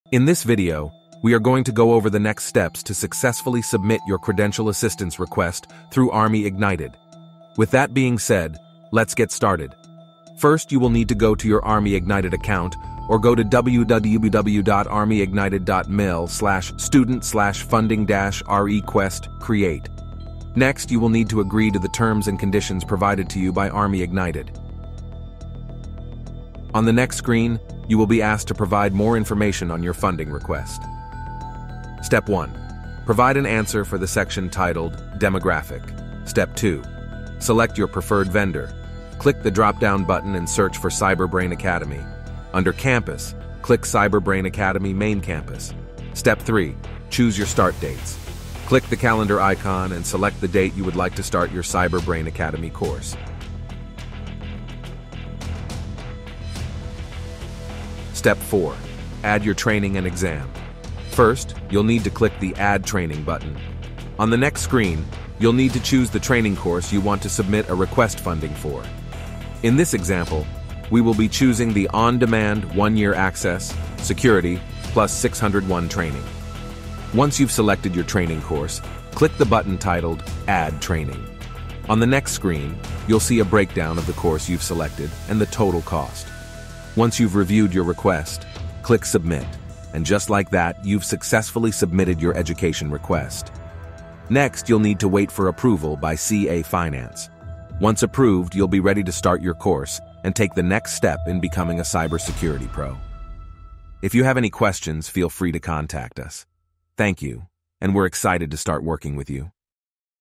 How to Submit Your Funding Request on ArmyIgnitED
How to request funding and choose a start date on Army Ignited 2023